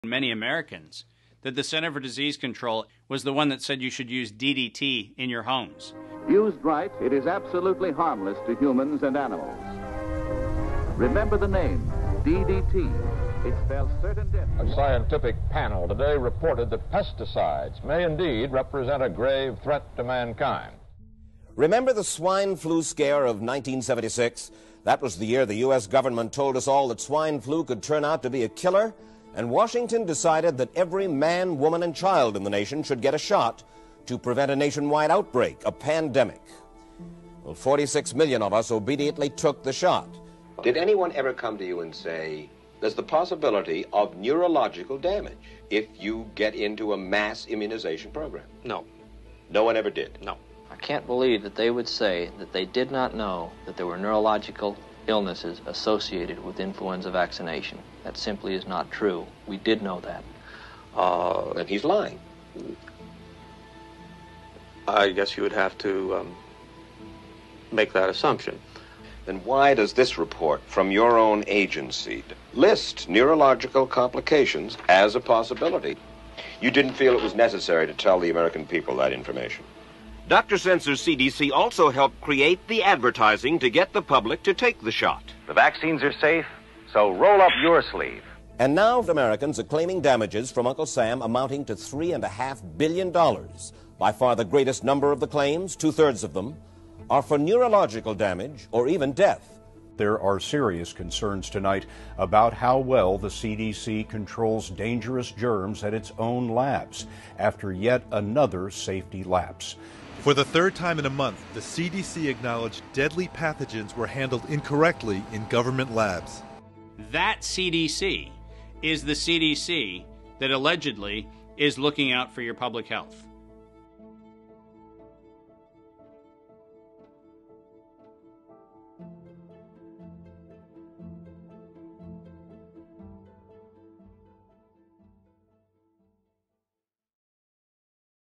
Povijest laganja
DDT apsolutno siguran za ljude i životinje! Cjepiva su sigurna, stoga zasučite rukave!

Ovo su marketinški slogani američkog CDC-a (Centar za kontrolu bolesti), državne institucije koja se navodno bavi zaštitom javnog zdravlja. I jedan i drugi slogan su čiste monstruozne laži koje su uzrokovale patnju i stradanja ljudi.

Tijekom navodne pandemije svinjske gripe 1976. godine, CDC i američka vlada su strahom i propagandom natjerali 46 miliona Amerikanaca da se cijepe – naravno, sa sigurnim i učinkovitim cjepivom. U videu možete vidjeti tadašnjeg direktora CDC-a kako bezočno laže da nije znao za moguću pojavu neuroloških bolesti nakon cijepljenja. Novinar mu pokazuje vlastito CDC izvješće koje govori o tome.
Znao je, kao što su i ostali sudionici u toj strahoti znali. Građanski zahtjevi za odštetama su se popeli do 3,5 milijarde dolara!

Globalisti i farma kartel su sada puno pametniji, jako su napredovali i u planiranju i u izvedbi. Reaganovim aktom iz 1986. su riješili pitanje odgovornosti farma kartela, zatim su marljivo potkupili političare, struku i medije. Možda je bolje reći kupili, jer je ta ekipa potpuno u njihovom vlasništvu. Tako da je covid prijevara lansirana besprijekorno!

Tri godine nakon najvećeg političko-medicinskog zločina u povijesti čovječanstva brojimo mrtve, ozlijeđene i slomljene ljude. Jedan dio ljudi i dalje živi u svojevrsnom hipnotičkom balonu, ne želeći se suočiti s istinom, jer je istina prebolna i preteška. Neki su od početka nanjušili prijevaru, neki se sad pomalo bude i uviđaju u kakvom svijetu žive.

Naši nacionalni heroji Beroš, Capak, Ceci, Bubašica…tri nas godine lažu i maltretiraju, odgovorni su za stradanja i patnju građana i nikome ništa. I dalje su tu, truju nas i dalje i poslušno izvršavaju svaki zadatak koji dobiju od svojih globalističkih šefova.